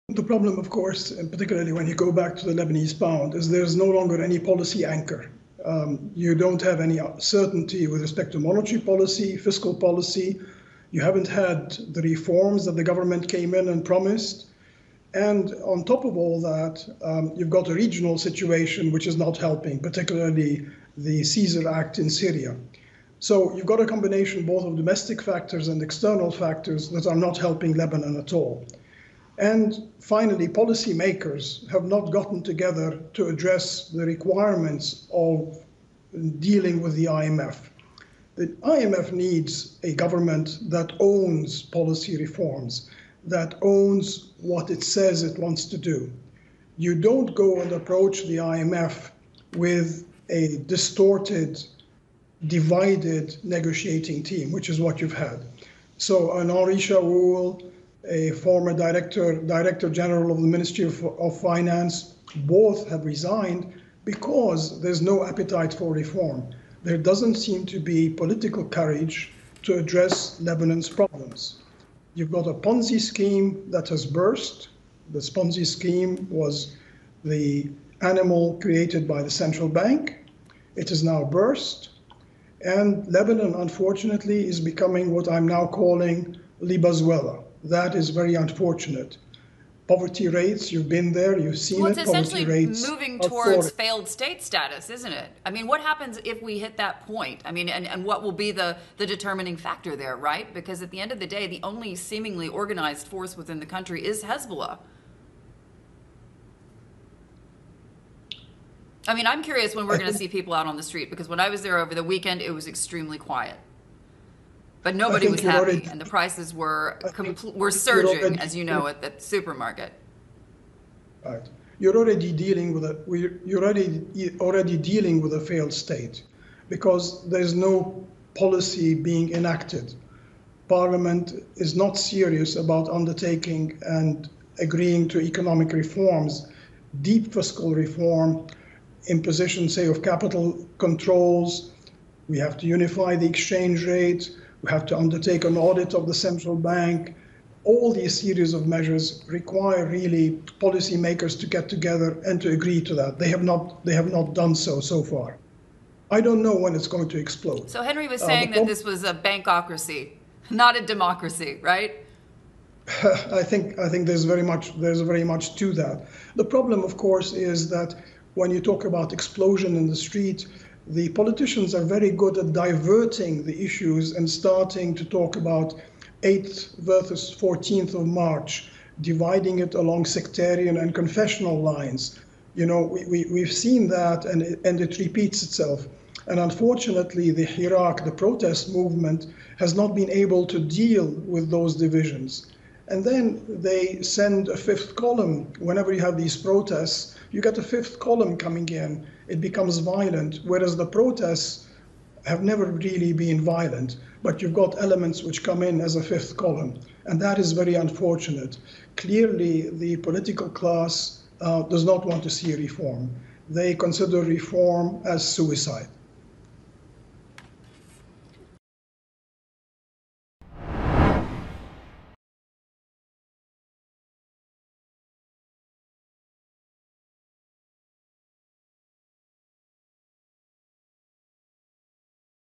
Lebanon is a 'failed state' with 'no appetite for reforms,' says former minister
Lebanon's leaders lack the "political courage" to address its problems, says Nasser Saidi, founder and president of Nasser Saidi & Associates, and former minister of economy and industry. He adds: "There's no policy being enacted. Parliament is not serious about undertaking and agreeing to economic reforms."